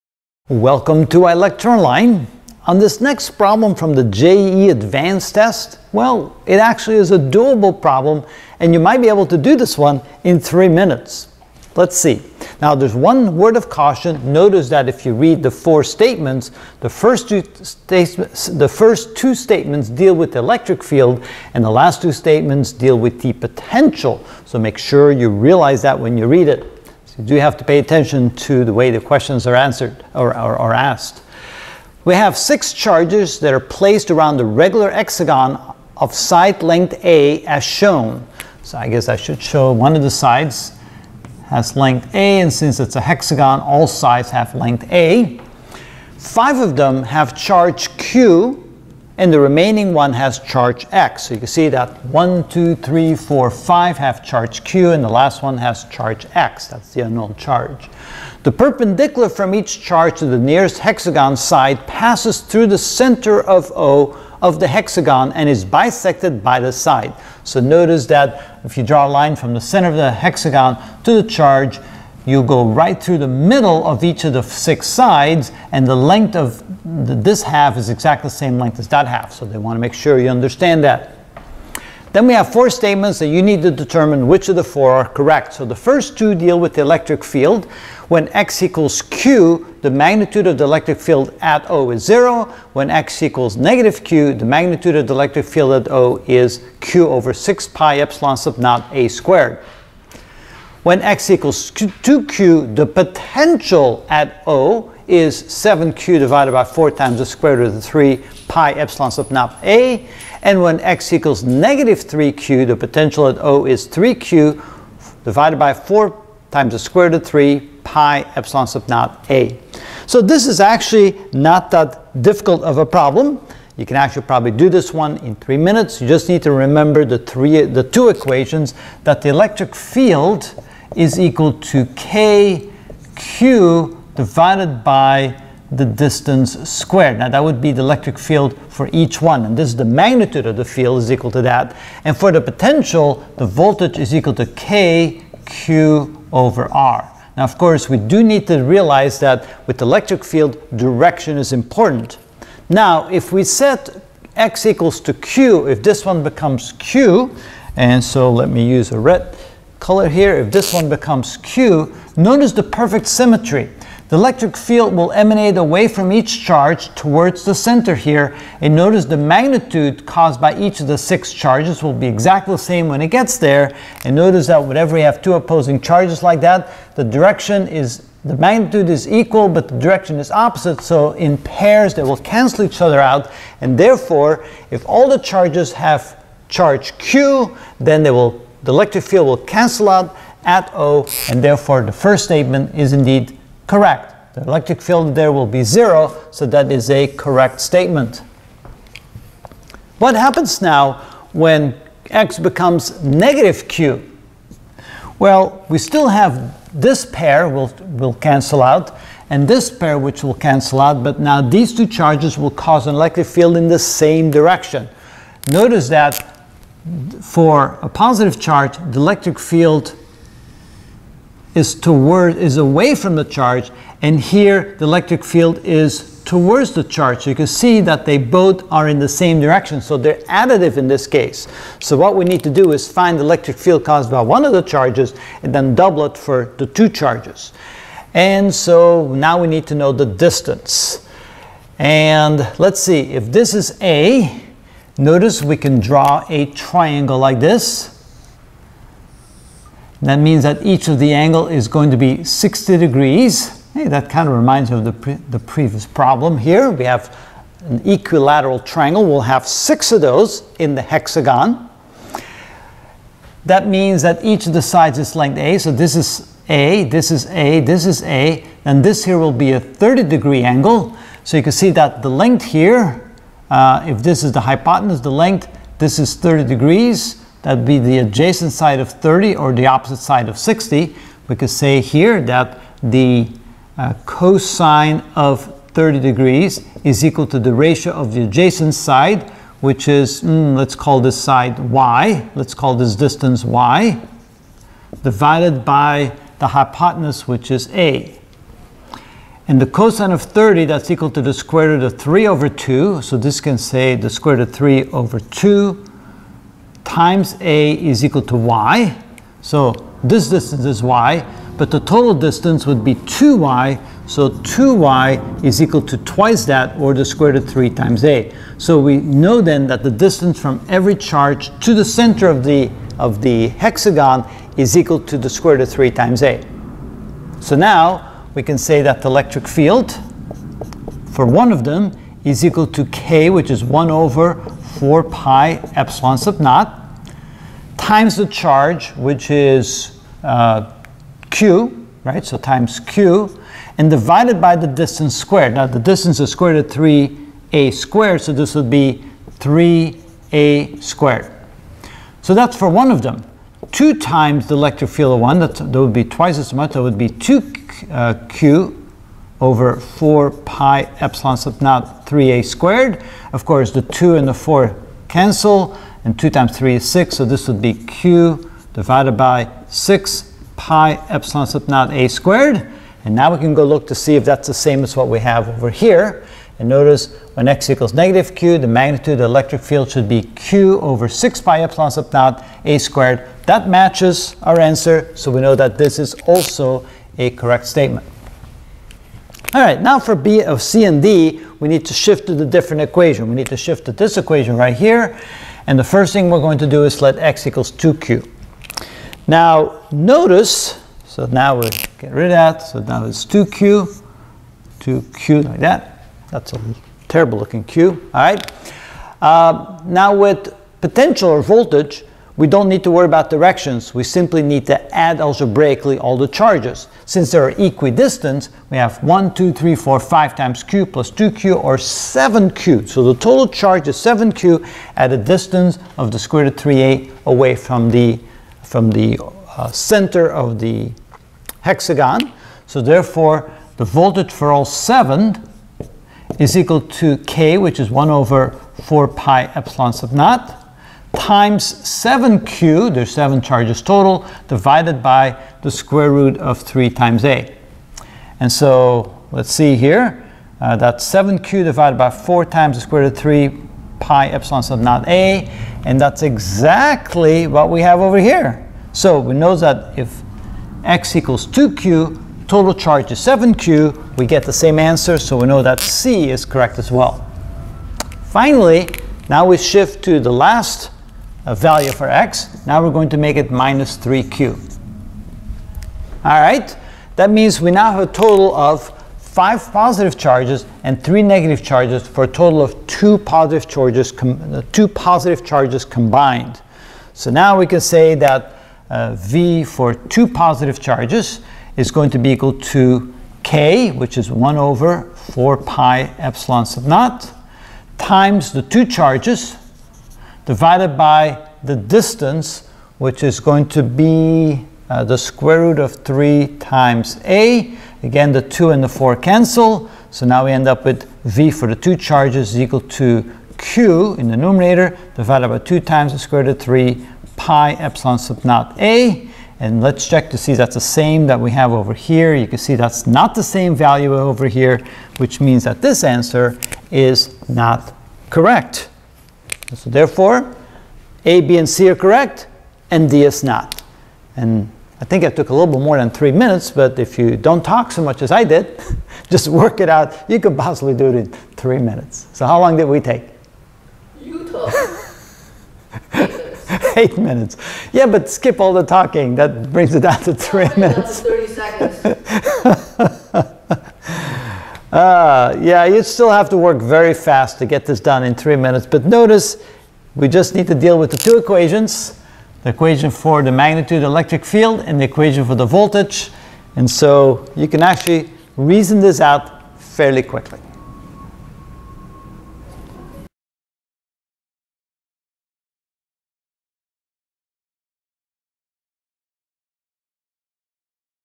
JEE Advanced Physics 2022 Paper 1: #13 Electric Field and Voltage
Six charges are placed around a regular hexagon of side length a as shown. Five of them have charge q, and the remaining one has charge x. The perpendicular from each charge to the nearest hexagon side passes through the center O of the hexagon and is bisected by the side. Which of the following statement(s) is (are) correct?
(A) When x=q the magnitude of the electric field at O is zero.
(B) When x=-q the magnitude of the electric field at O is q/[6(pi)(epsilon0)(a^2)].
(D) When x=-3q the potential at 0 is 3q/[4(SQRT(3))(pi)(epsilon0)a].

Title seach on other plateforms:  JEE Advanced Physics 2022 Paper 1: #12 Reflection with Mirrors
Title search on other plateforms:  JEE Advanced Physics 2022 Paper 1: #14 The Binding Energy